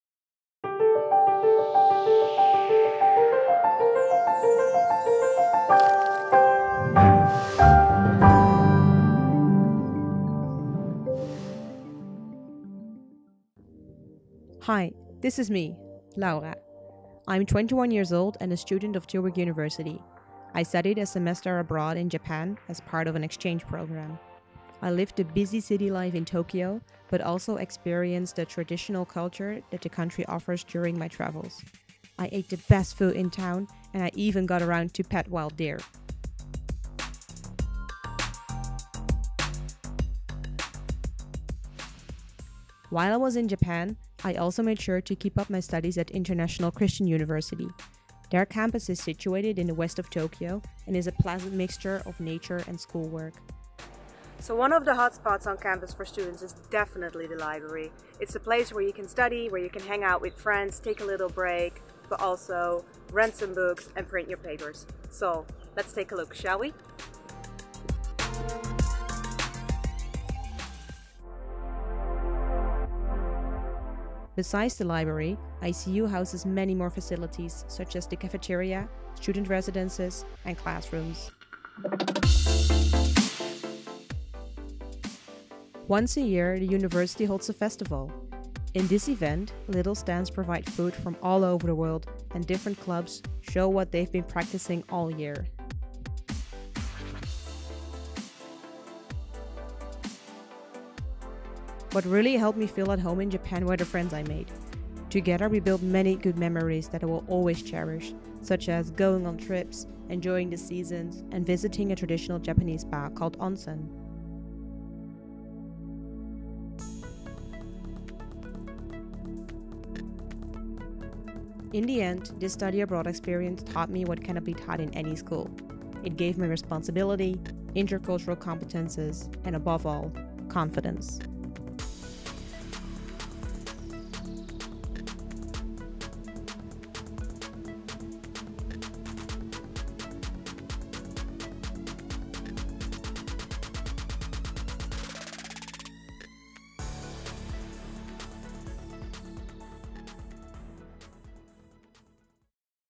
SAVE - University (ICU) in Tokyo, Japan - Fall 2016 - International Office - Tilburg University
Study Abroad Video Experience (SAVE). This video is made by an exchange student of Tilburg University during her stay at University (ICU) Tokyo in Japan (Fall 2016).